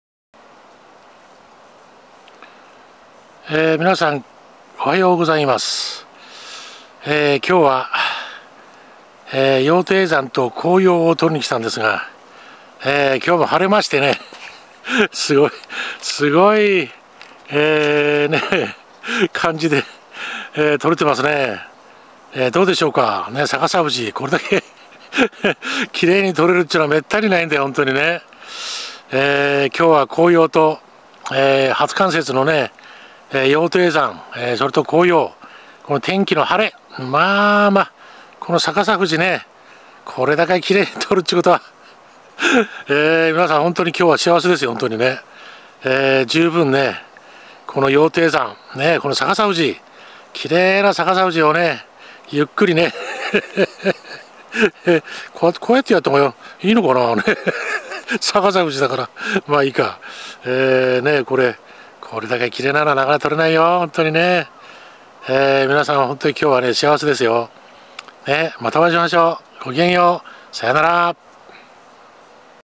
【北海道観光】「羊蹄山の逆さ富士」と「黄葉」＝ 北海道の絶景
「エゾ富士の逆さ富士」羊蹄山の逆さ富士